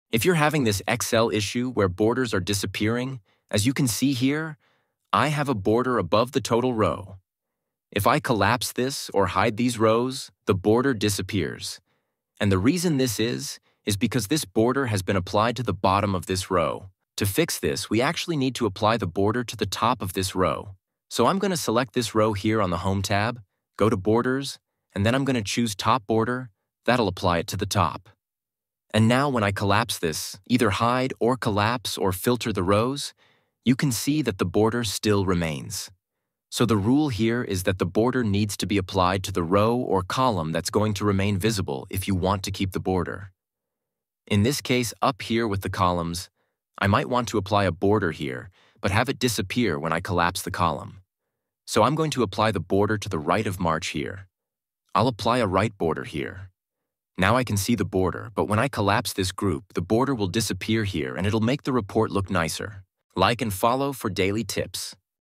Why Do Cell Borders Disappear in Excel? | Quick Fix Explained!
Are your cell borders disappearing in Excel, leaving your spreadsheets looking incomplete or messy? In this video, we explain why this happens and how you can fix it quickly. Whether it’s due to print settings, zoom levels, or formatting issues, we’ve got you covered with step-by-step solutions to make your borders visible again.

Key Takeaways:

Common reasons for disappearing borders.
Adjusting print settings to retain borders.
Formatting tips to ensure consistent borders.
📊 Say goodbye to disappearing cell borders and create polished, professional spreadsheets!